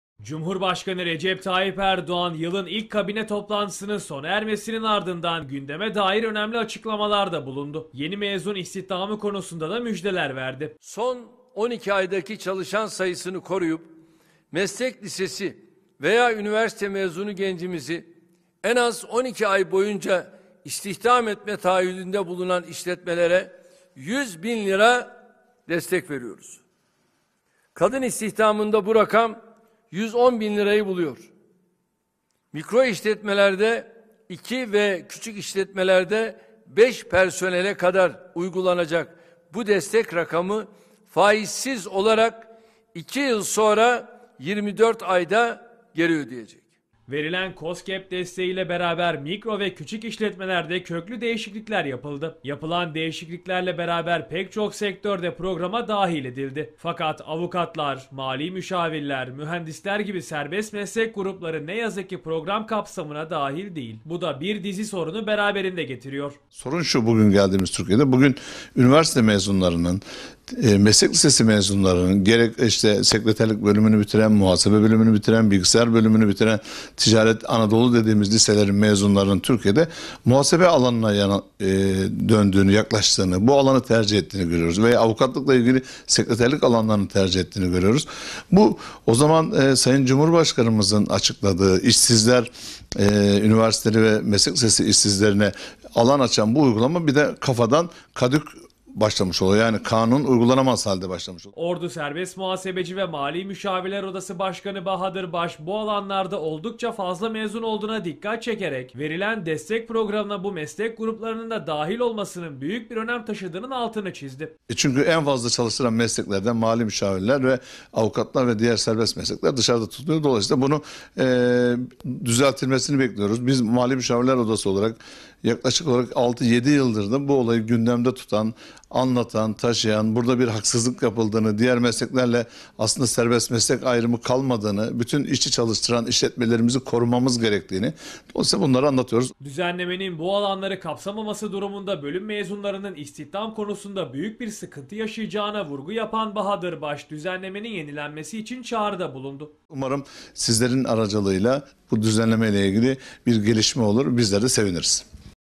Oda Başkanımız Bahadır BAŞ; KOSGEB Programlarının Yeniden Düzenlenmesi Gerektiğini TV52'ye Açıkladı.
Oda Başkanımız Bahadır Baş; Hızlı Destek Programı Kapsamında Mikro ve Küçük İşletmelere Sağlanan KOSGEB Programlarının Yeniden Düzenlenerek Serbest Meslek Mensuplarının da Dahil Edilmesi Gerektiğini TV52’ye Değerlendirdi.